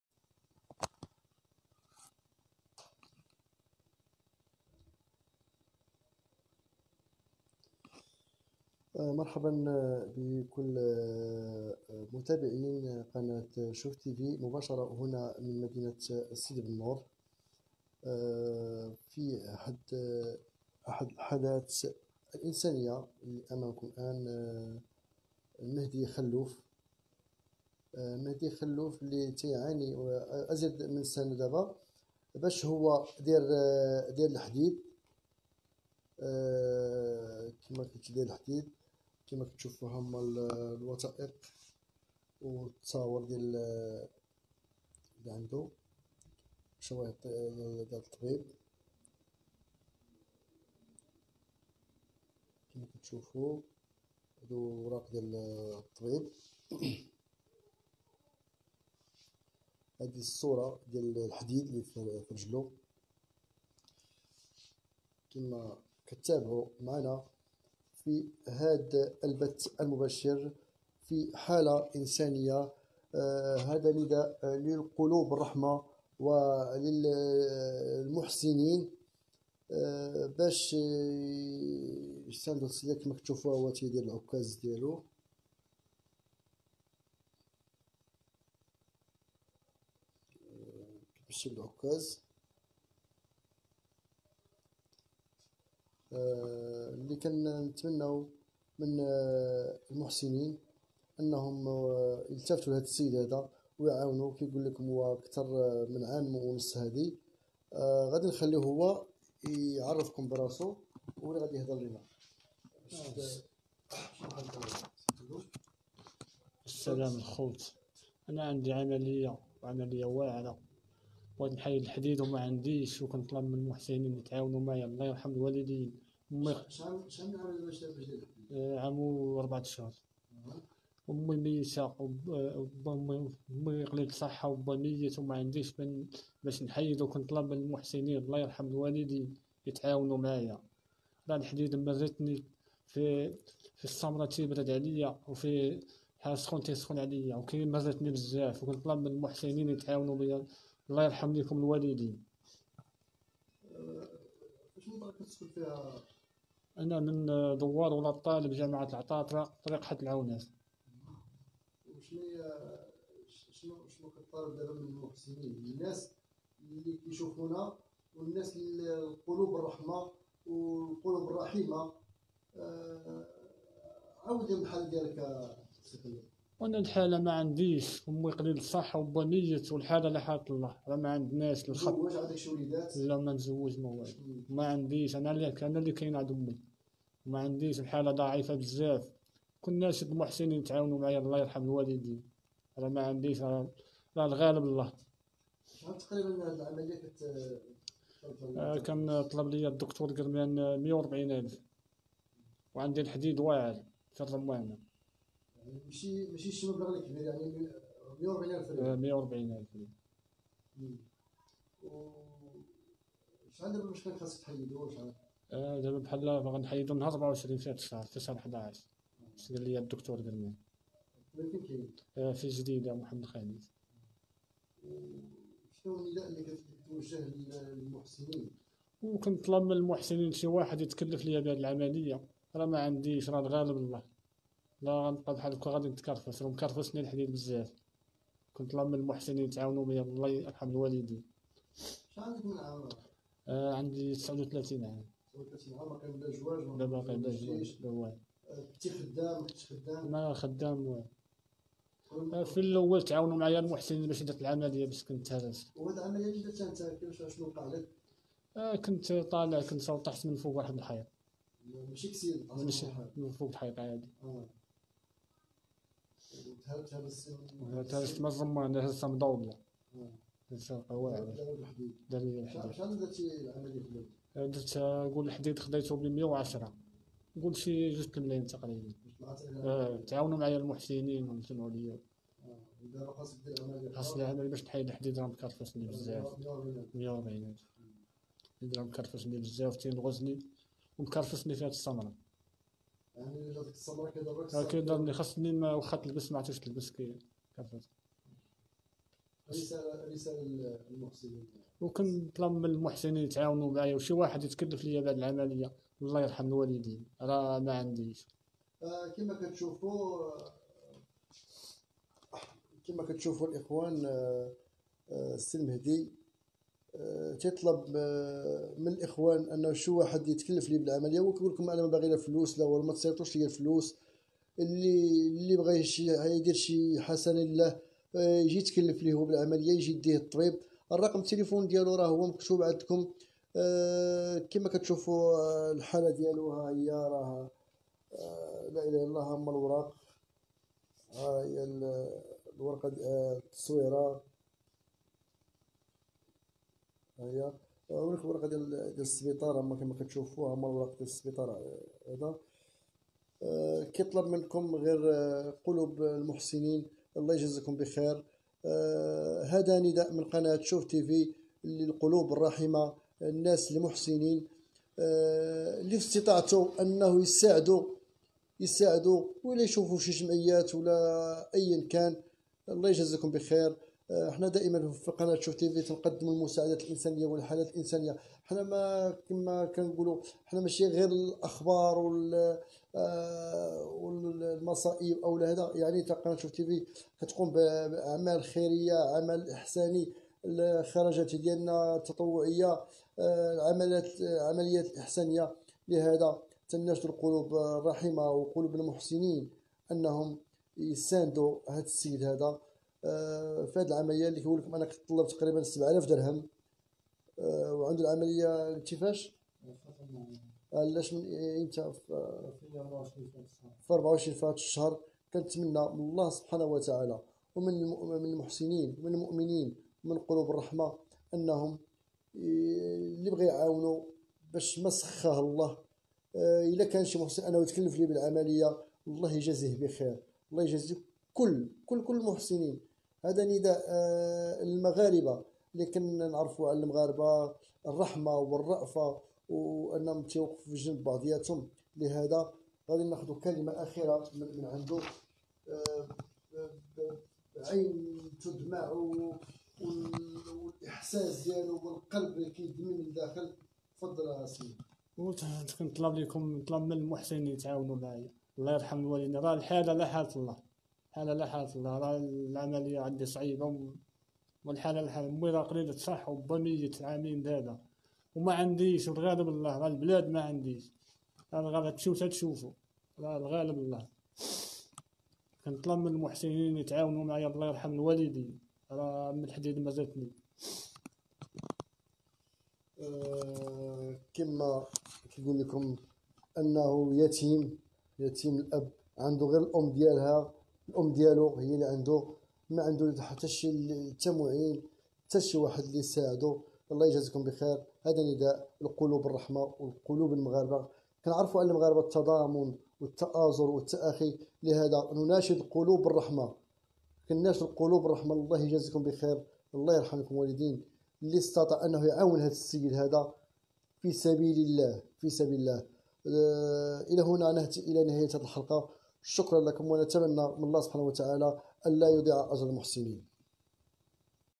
مواطن داير الحديد فرجليه هادي كثر من عام ونص وماعندوش الفلوس باش يدير العملية ومحتاج للمحسنين
مباشرة من سيدي بنور: مواطن داير الحديد فرجليه هادي كثر من عام ونص وماعندوش الفلوس باش يدير العملية ومحتاج للمحسنين يوقفو معاه ...للمساعدة : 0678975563